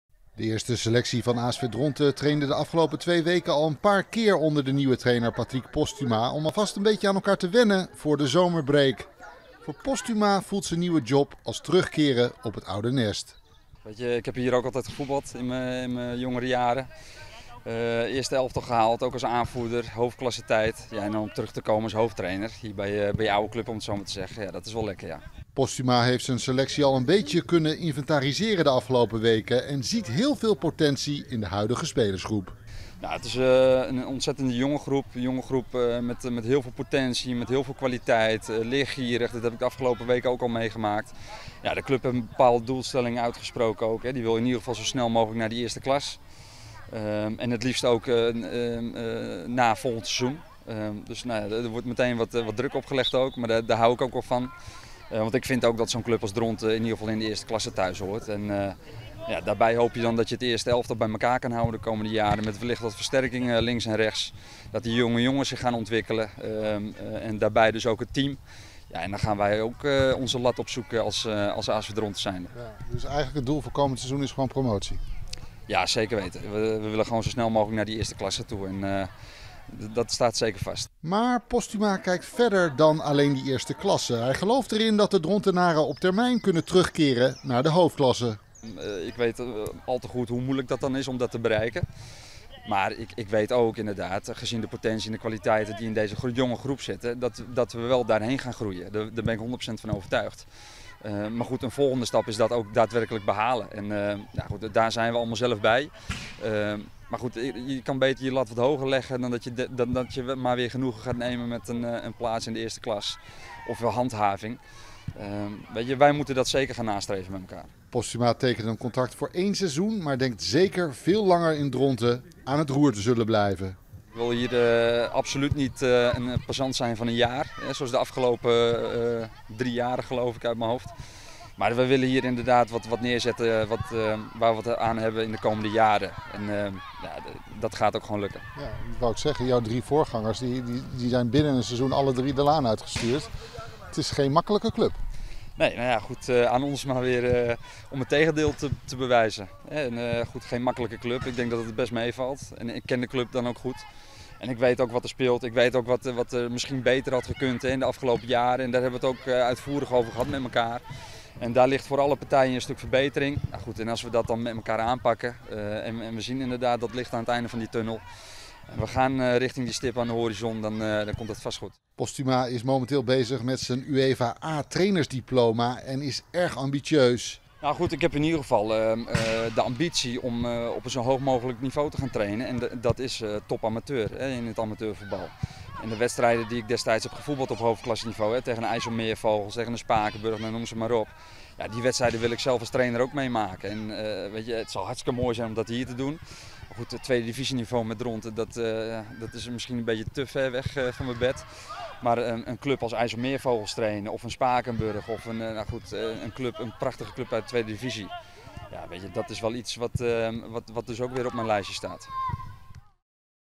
Posthuma wil asv Dronten terugbrengen naar Hoofdklasse
De nieuwe hoofdtrainer van ASV Dronten Patrick Posthuma wil komend seizoen met zijn club promoveren naar de eerste klasse. Posthuma wil op langere termijn de Drontenaren gaan terugbrengen naar de Hoofdklasse.